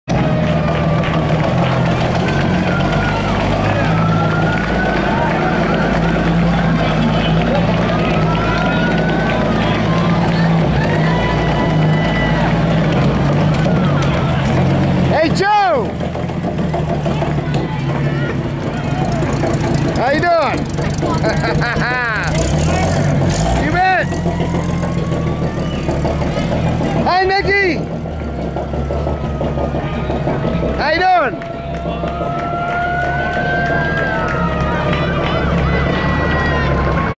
CLUW Ladder Truck.mpg
Coalition of Labor Union Women (CLUW), Service Employees Internation Union (SEIU) Local 1021 aqnd the San Joaquin/Calaveras Central Labor Council participate in Stockton's 4th of July Parade down Miracle Mile riding in a Stockton Fire Department Ladder Truck